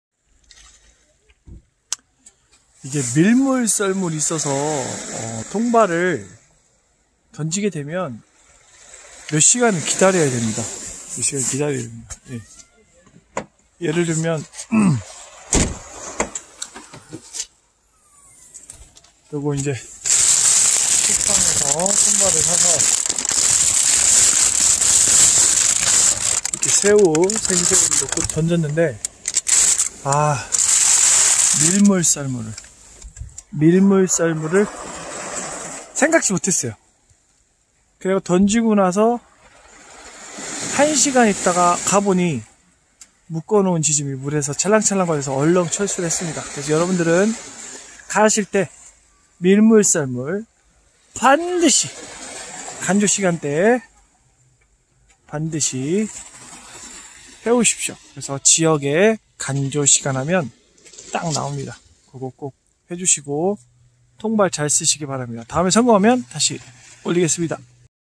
바닷가 통발 설치시 주의사항
간조, 만조 시간을 꼭 조사하세요.
오늘은 실패 ㅜ